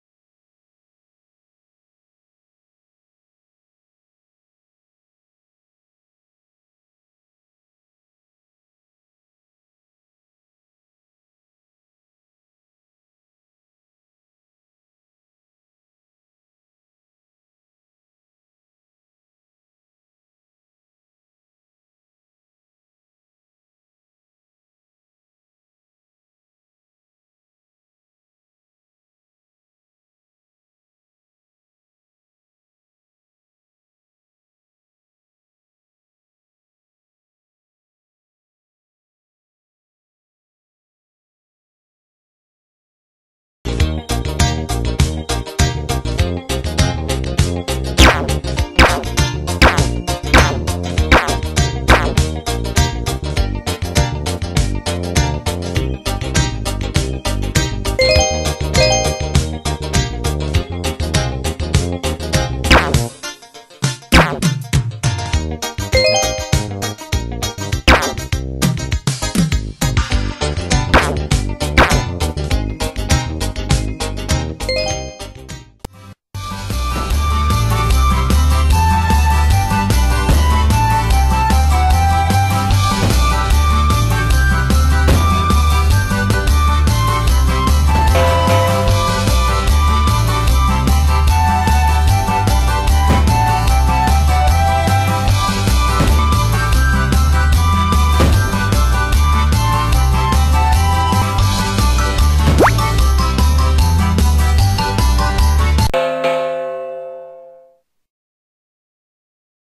Multi-mouse Games
Five educational games that allow you to use multi mouse on a single computer.  These were built using mutlipoint sdk and silverlight. Two games were built in popfly.